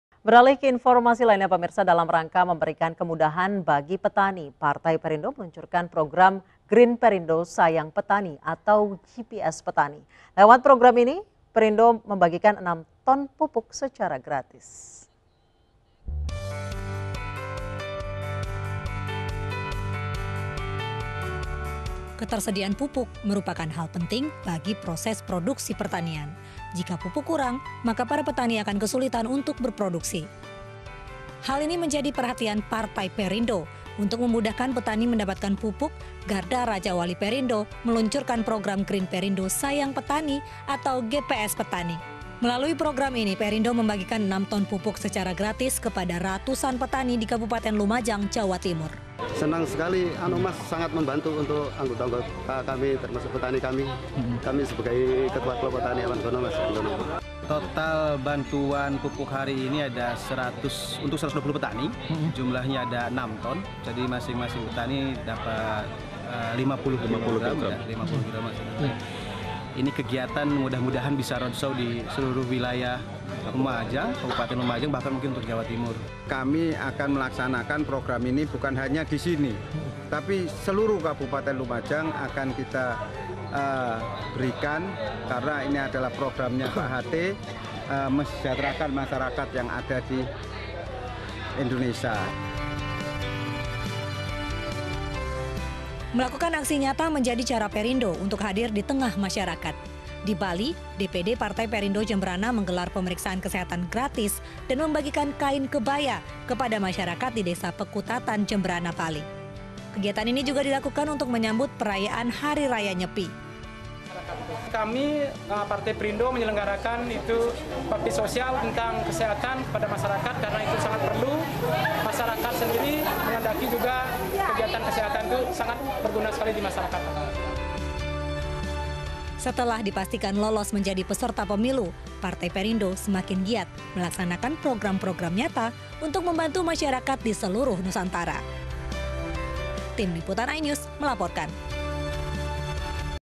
Pastikan Ketersediaan Pupuk, Perindo Salurkan 6 Ton Pupuk ke Petani Lumajang
Di Kabupaten Lumajang, Jawa Timur, guna memastikan ketersediaan pupuk, Partai Perindo melalui organisasi sayap Garda Rajawali Perindo (GRIND) membagikan setidaknya 6 ton pupuk ke 120 petani di Kecamatan Kunir, Lumajang, Jumat (9/3). Tidak hanya di Kecamatan Kunir, pupuk gratis ke petani akan juga dibagikan daerah lain di Kabupaten Lumajang.